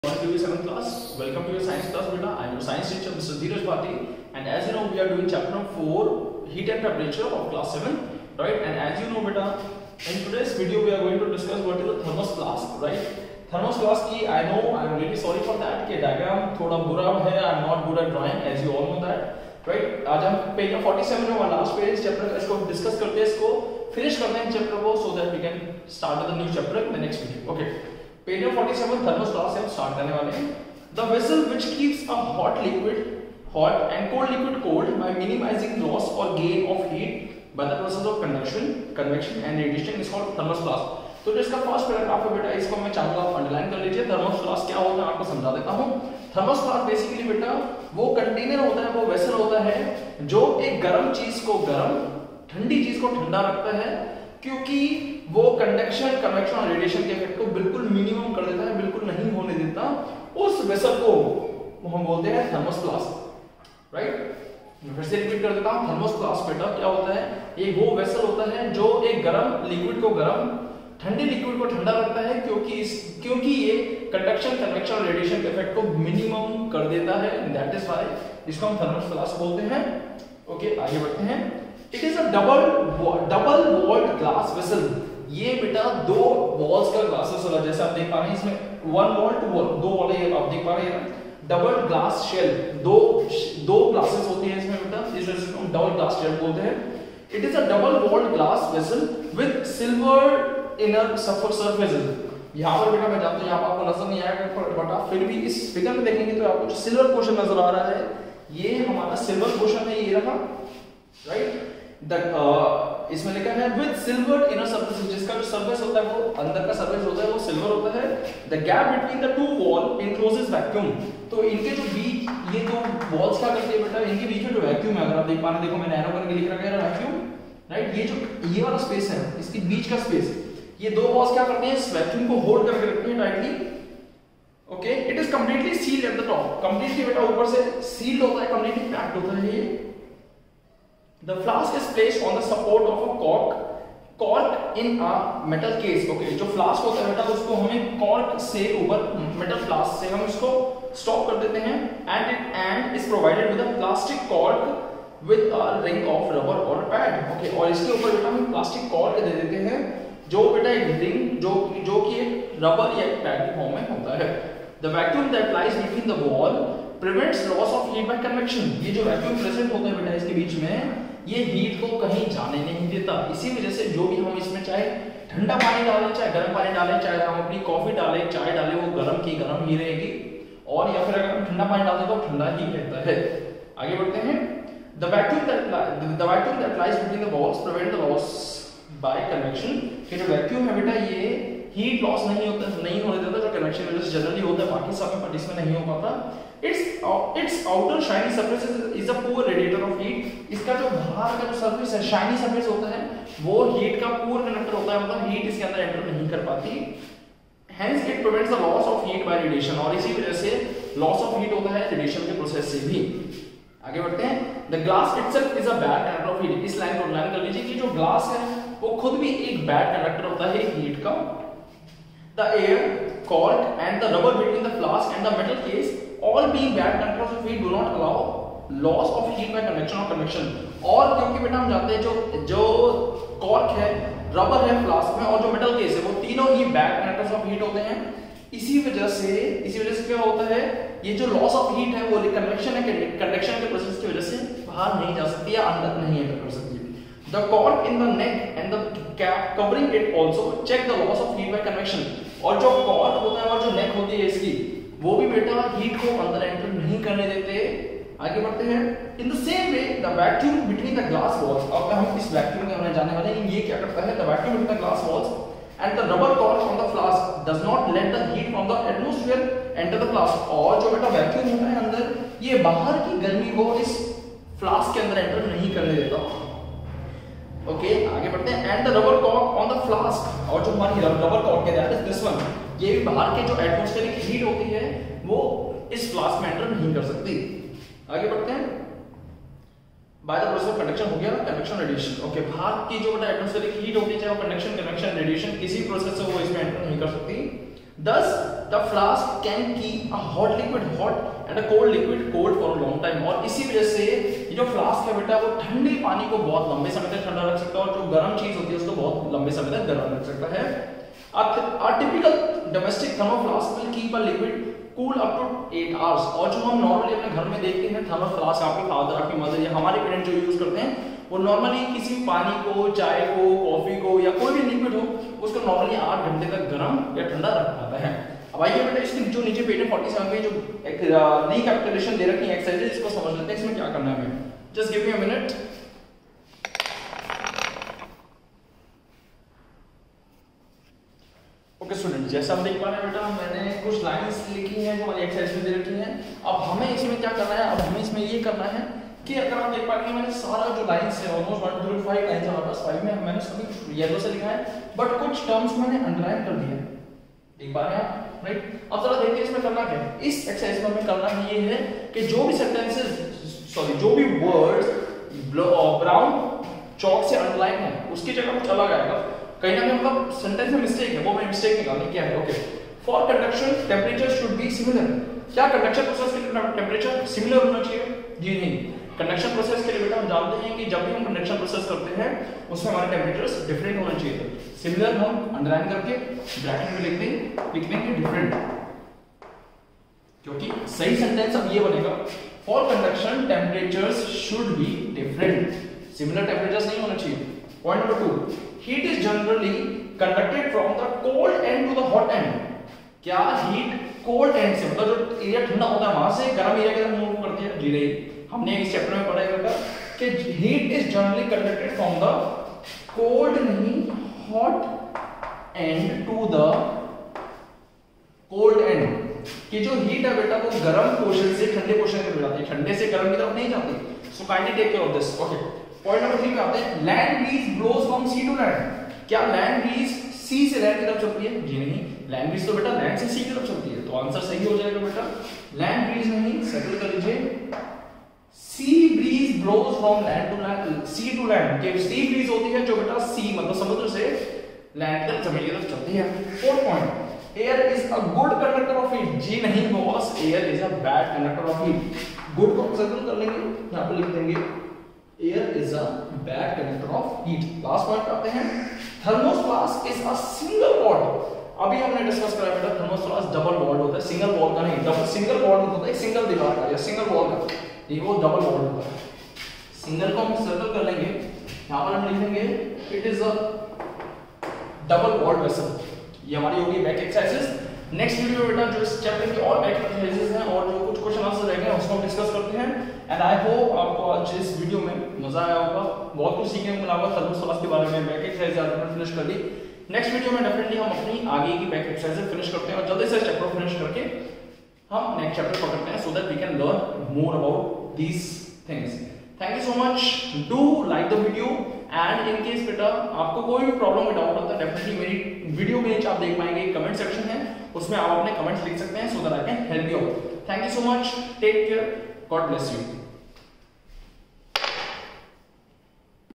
Class -7 Science Ch-4 (Heat and Temperature)
Topic - Thermos Flask
Dated - 17/08/2020
By- Dheeraj Bhati